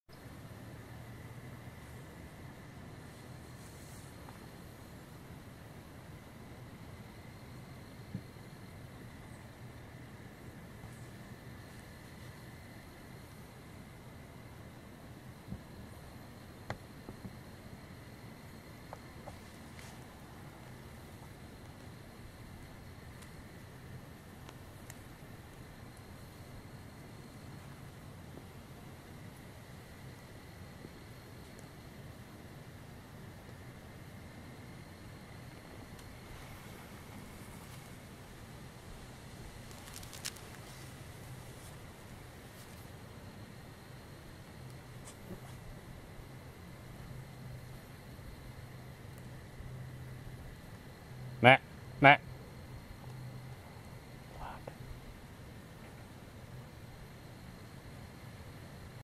2017 Almost Connect Bowhunt
minnesota bowhunt 2017, First doe sighting, raining day bowhunt, almost connect about 35 yards